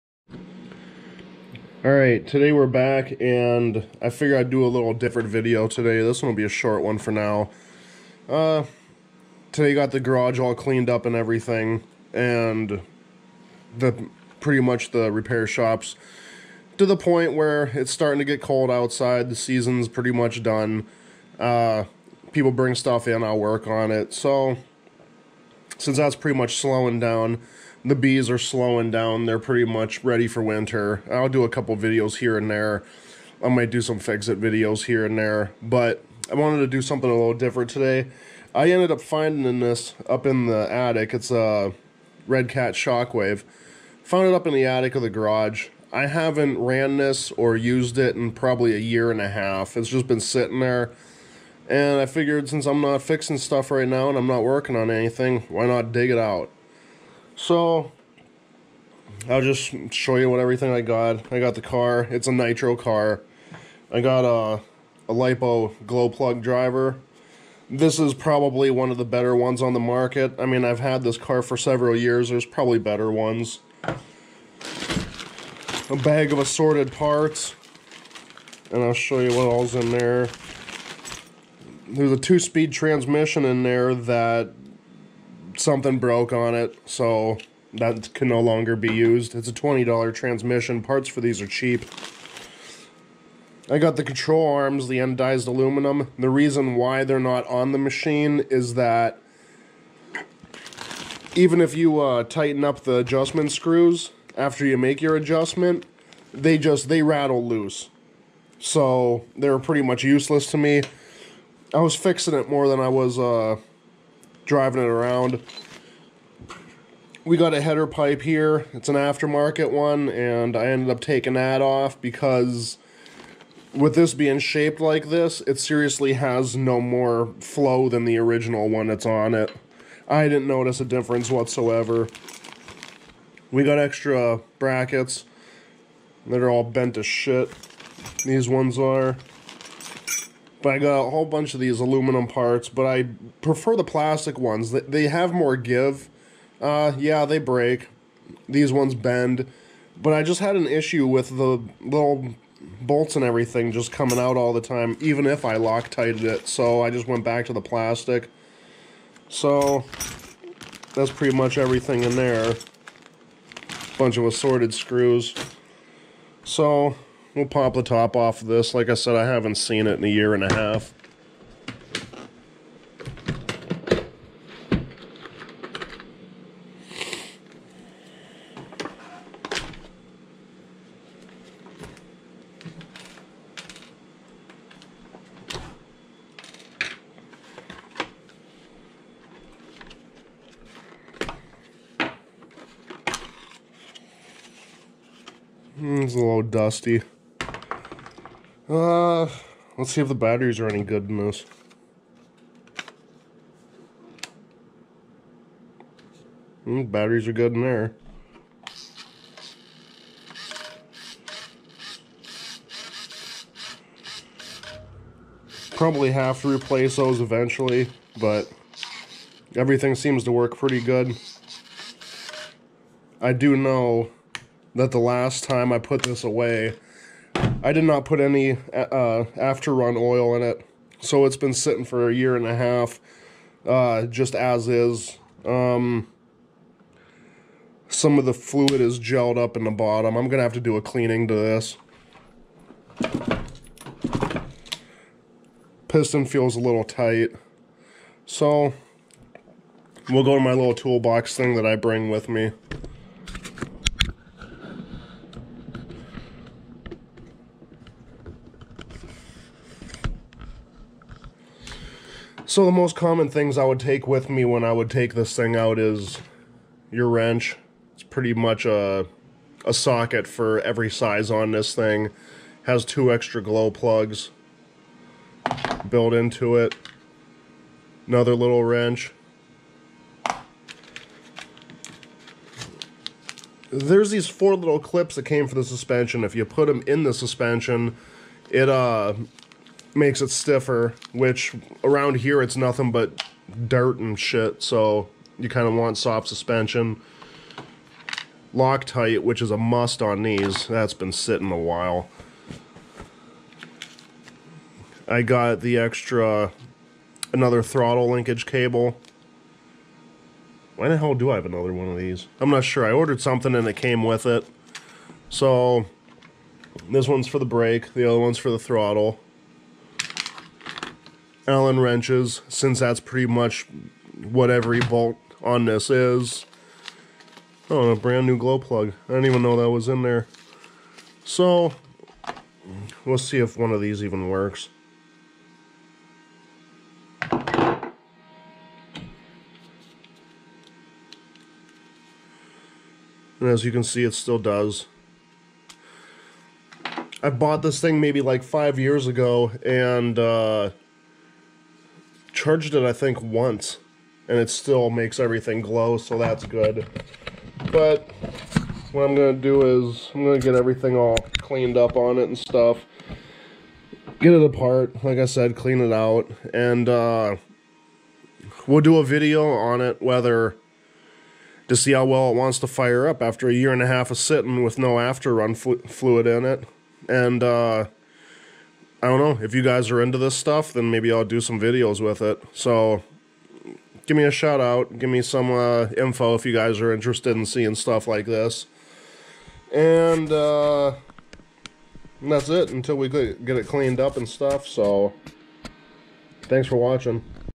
Nitro RC buggy found after a year and a half
i forgot all about this nitro rc. by the way i have never put after run oil in it ever!